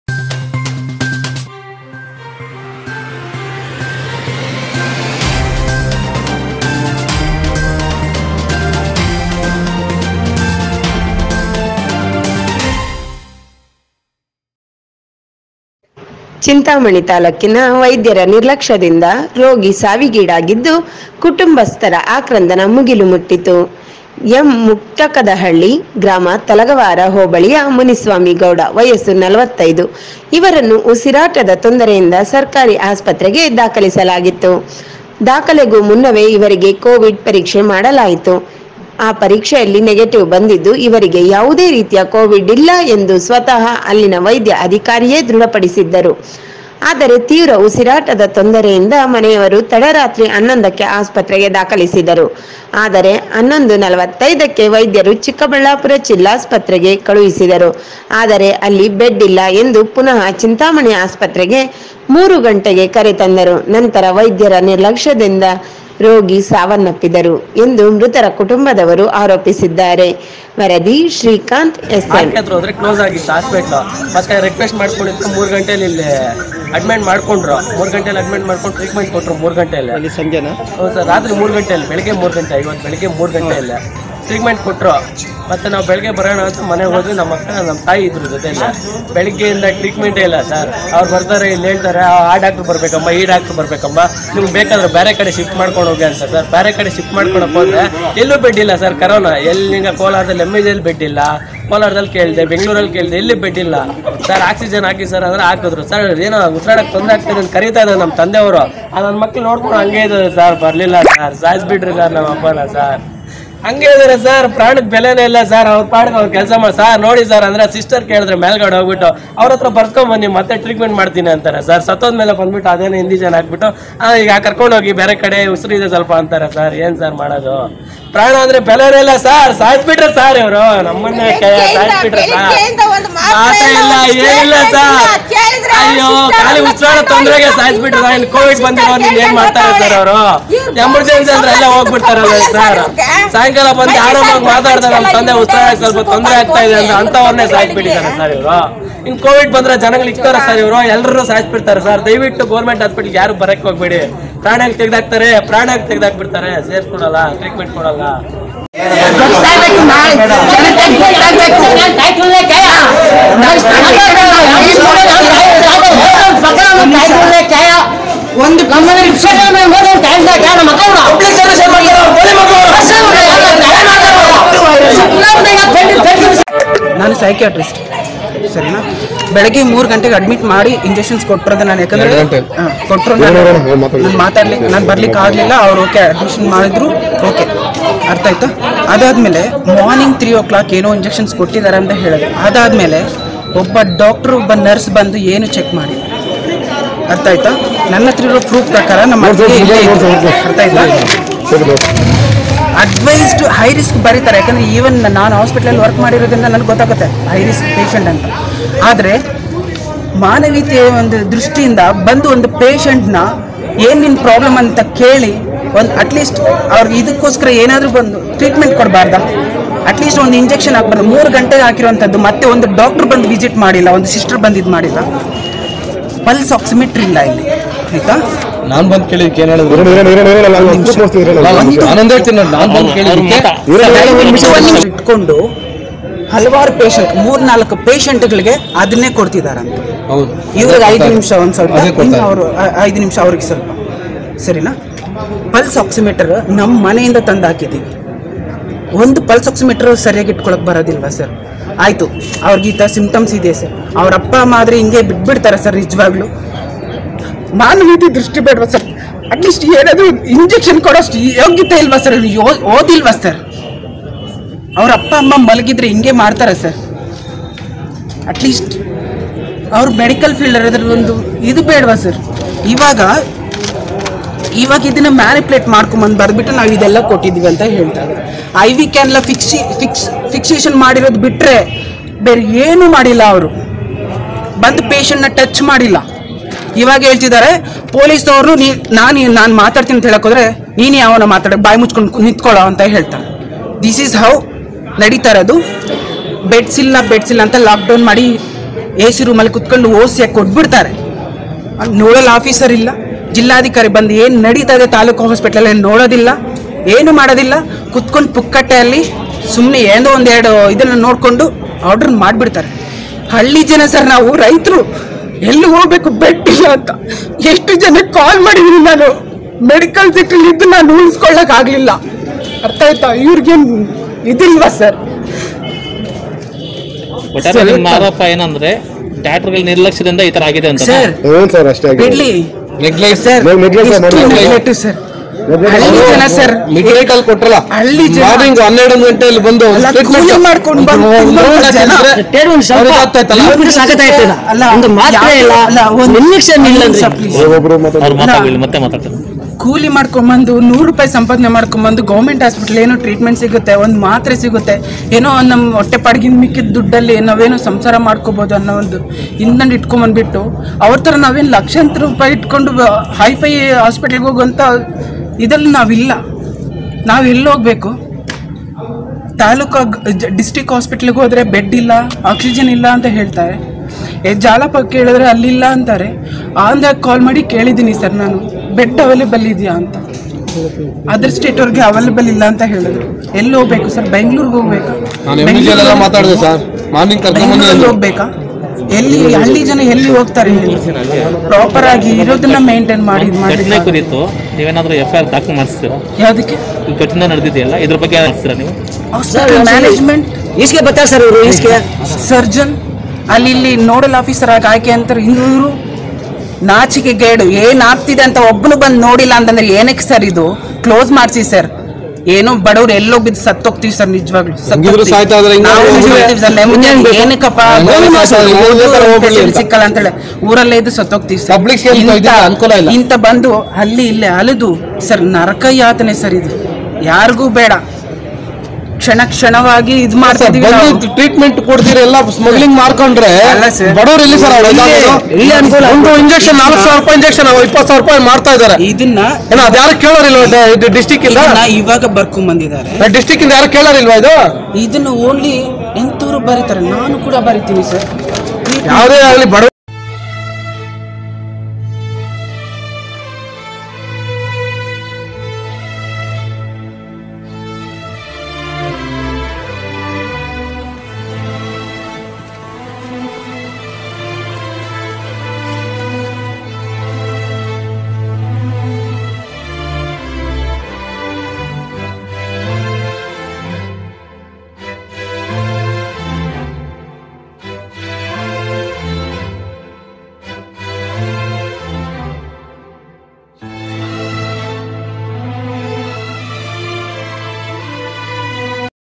ವೈದ್ಯರ ನಿರ್ಲಕ್ಷ್ಯದಿಂದ ರೋಗಿ ಸಾವು
ಚಿಂತಾಮಣಿ : ವೈದ್ಯರ ನಿರ್ಲಕ್ಷ್ಯದಿಂದ ರೋಗಿ ಸಾವಿಗೀಡಾಗಿದ್ದು, ಕುಟುಂಬಸ್ತರ ಆಕ್ರಂದನ ಮುಗಿಲು ಮುಟ್ಟಿತ್ತು. ಎಂ.ಮುಟ್ಟಕದಳ್ಳಿ (ಗ್ರಾಮ) ತಲಗವಾರ ಹೋಬಳಿಯ ಮುನಿಸ್ವಾಮಿಗೌಡ ವಯಸ್ಸು 45  ಇವರನ್ನು ಉಸಿರಾಟದ ತೊಂದರೆಯಿಂದ ಸರ್ಕಾರಿ ಆಸ್ಪತ್ರೆಗೆ ದಾಖಲಿಸಲಾಗಿತ್ತು. ದಾಖಲೆಗೂ ಮುನ್ನವೇ ಇವರಿಗೆ ಕೋವಿಡ್ ಪರೀಕ್ಷೆ ಮಾಡಲಾಯಿತು. ಆ ಪರೀಕ್ಷೆಯಲ್ಲಿ ನೆಗಟಿವ್ ಬಂದಿದ್ದು, ಇವರಿಗೆ ಯಾವುದೇ ರೀತಿಯ ಕೋವಿಡ್ ಇಲ್ಲ ಎಂದು ಸ್ವತಃ ಅಲ್ಲಿನ ವೈದ್ಯ ಅಧಿಕಾರಿಯೇ ದೃಢಪಡಿಸಿದರು. ಆದರೆ ತೀವ್ರ ಉಸಿರಾಟದ ತೊಂದರೆಯಿಂದ ಮನೆಯವರು ತಡರಾತ್ರಿ 11ಕ್ಕೆ ಆಸ್ಪತ್ರೆಗೆ ದಾಖಲಿಸಿದರು. ಆದರೆ 11:45 ಕ್ಕೆ ವೈದ್ಯರು ಚಿಕ್ಕಬಳ್ಳಾಪುರ ಜಿಲ್ಲಾಸ್ವತ್ರೆಗೆ ಕಳುಹಿಸಿದರು. ಆದರೆ ಅಲ್ಲಿ ಬೆಡ್ ಇಲ್ಲ ಎಂದು ಪುನಃ ಚಿಂತಾಮಣಿ ಆಸ್ವತ್ರೆಗೆ 3 ಗಂಟೆಗೆ ಕರೆತಂದರು ನಂತರ ವೈದ್ಯರ ನಿರ್ಲಕ್ಷ್ಯದಿಂದ ರೋಗಿ ಸಾವನ್ನಪ್ಪಿದರು ಎಂದು ಮೃತರ ಕುಟುಂಬದವರು ಆರೋಪಿಸಿದ್ದಾರೆ.
ವರದಿ : ಶ್ರೀಕಾಂತ್ ಎಸ್ ಎಮ್